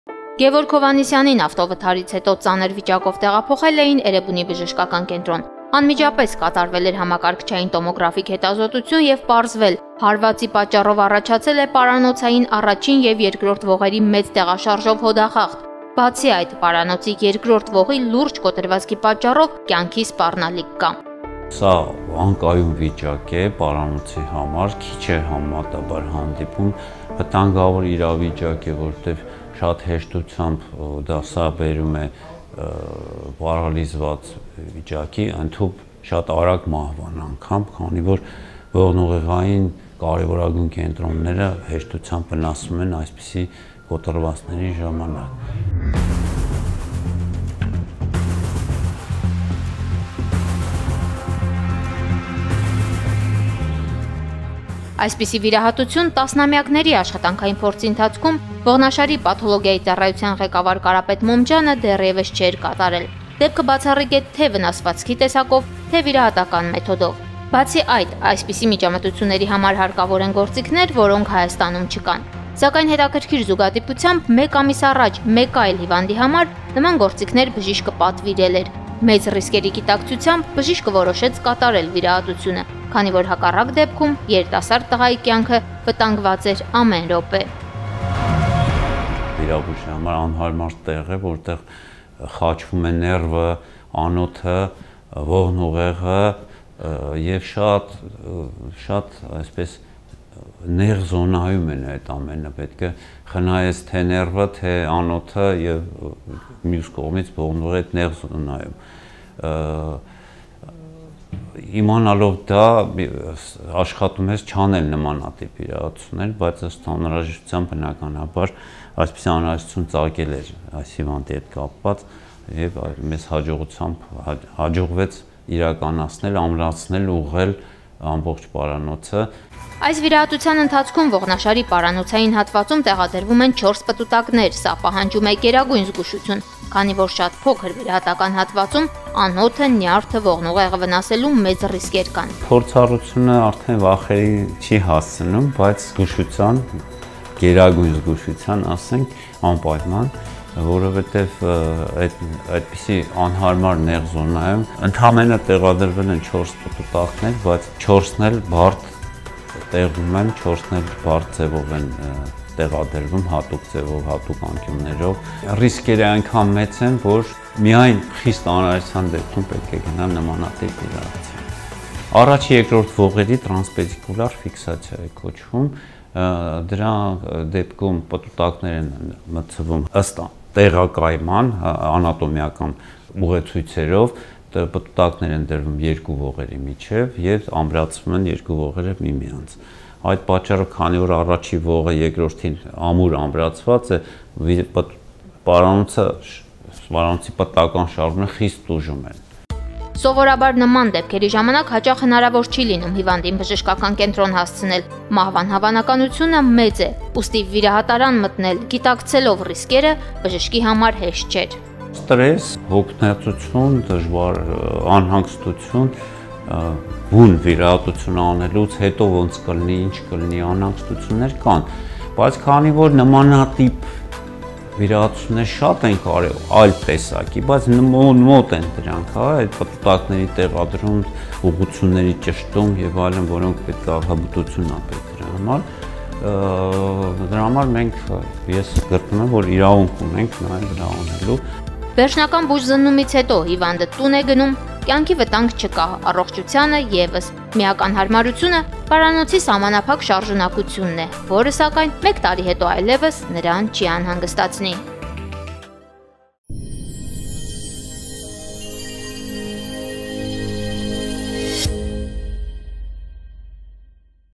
Ողնաշարի բացառիկ վիրահատություն է կատարվել ավտովթարից հետո
Ողնաշարի բացառիկ վիրահատություն է կատարվել  ավտովթարից հետո Էրեբունի ԲԿ-ում:   Ավտովթարից հետո երիտասարդ տղայի մոտ առաջացել էր պարանոցային առաջին և երկրորդ  ողերի մեծ տեղաշարժով հոդախախտ,  բացի այդ, պարանոցի երկրորդ  ողի լուրջ  կոտրվածք կար, ինչն ամեն րոպե կարող էր կյանքի սպառնալիք դառնալ:  Հաշվի առնելով բոլոր ռիսկերը՝ «Էրեբունի» ԲԿ ողնաշարի ախտաբանության ծառայության ղեկավար Կարապետ Մոմջյանը կատարել է ողնաշարի առաջին և երկրորդ ողերի տրանսպետիկուլյար ֆիքսացիա: Երիկտասարդ տղայի կյանքը փրկվել է:

🏥Հետեւեք մեզ նաև
🏥Երևան, Տիտոգրադյան 14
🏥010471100